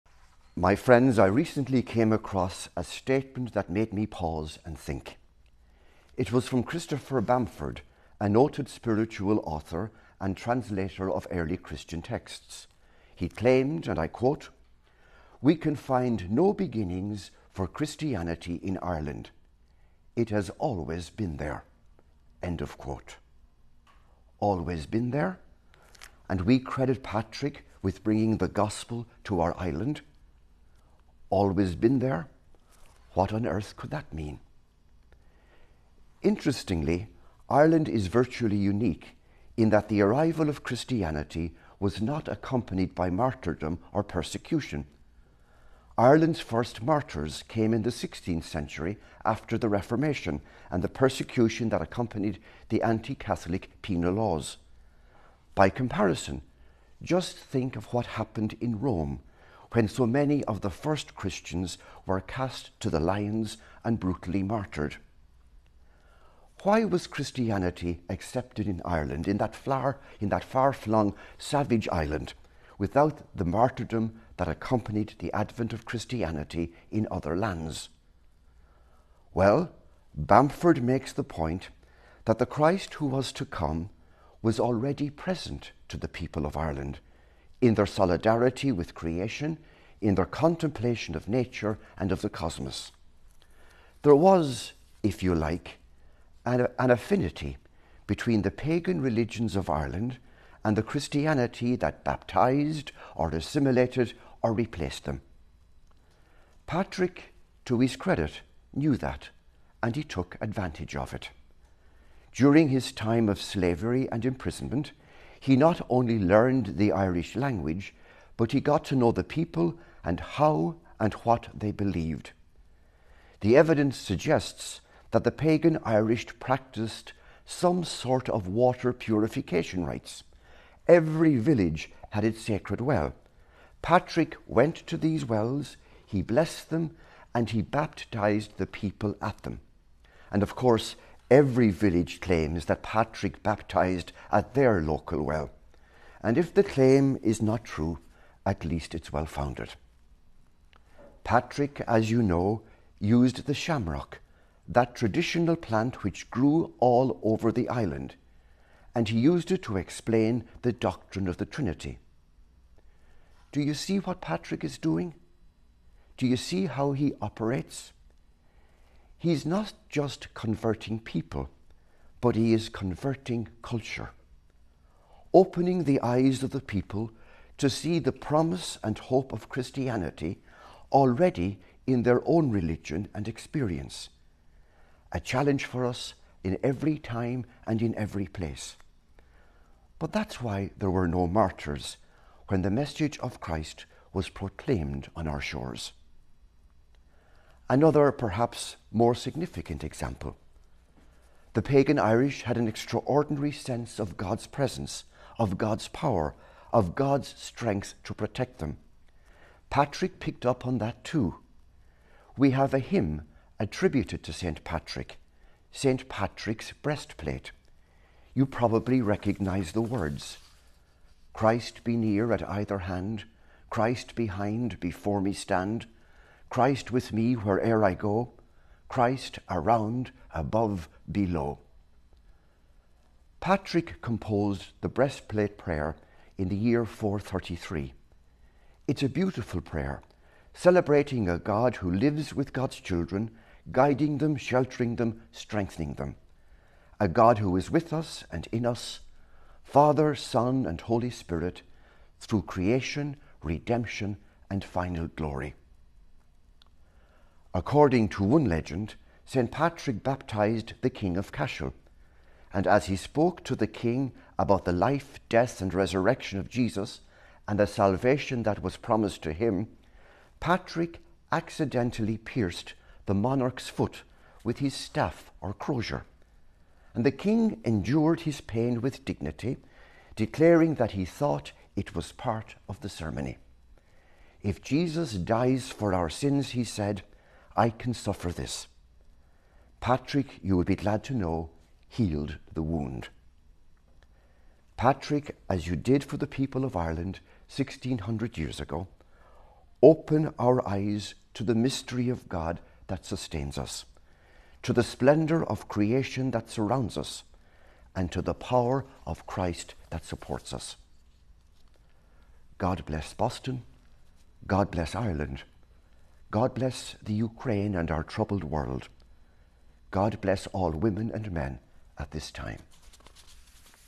St. Patrick | Homily: Monsignor Liam Bergin
Monsignor Liam Bergin tells us the story of St. Patrick, and the reason for his success when he evangelized Ireland.